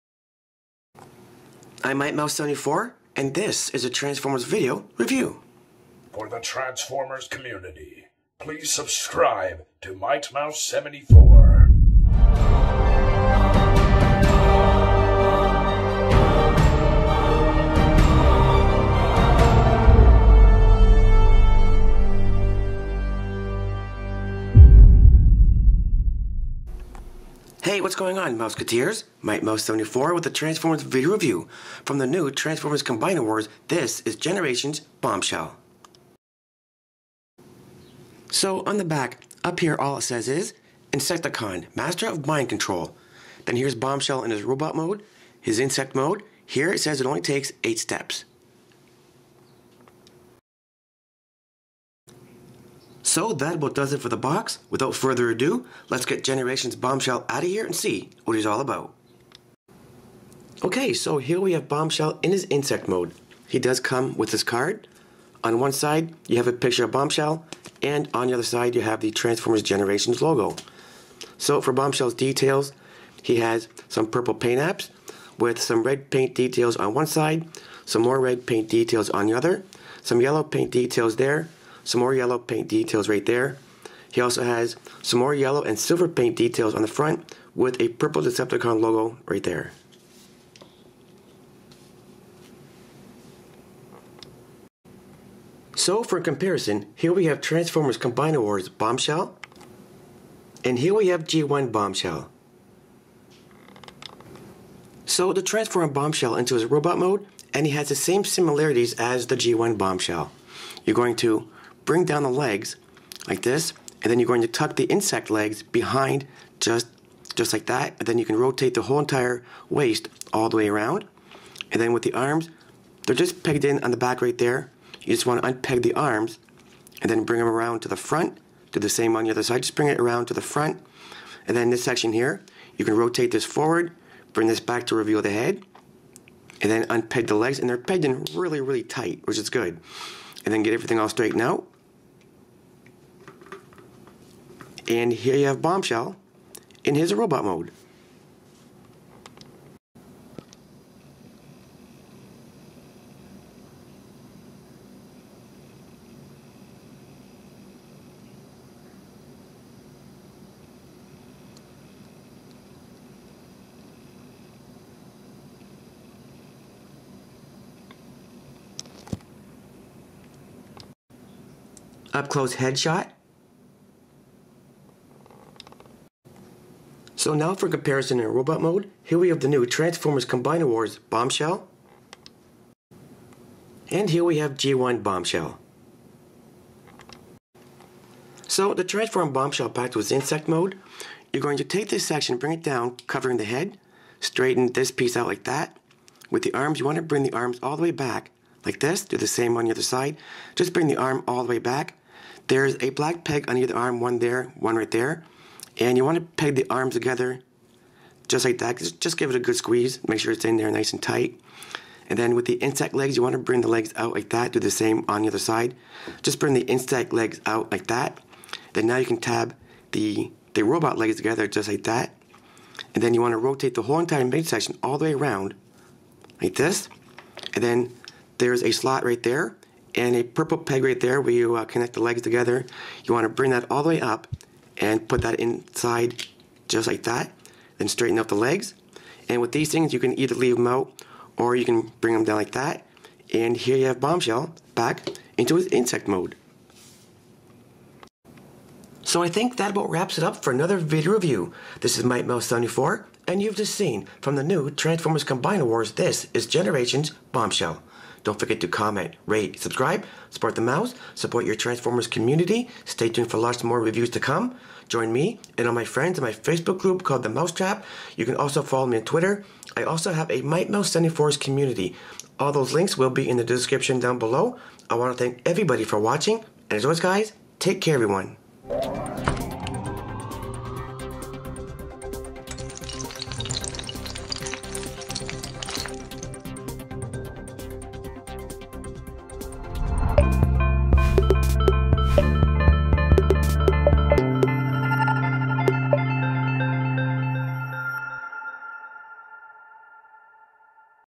TRANSFORMERS COMBINER WARS GENERATIONS BOMBSHELL TOY REVIEW
Hey everyone...here is my video review of the new Transformers Combiner Wars Generations Bombshell! In the comments below...let me know what you think! Thanxs for watching guys! =D
Come hang out with me & all my friends on my Facebook group The Mouse Trap! here's the link: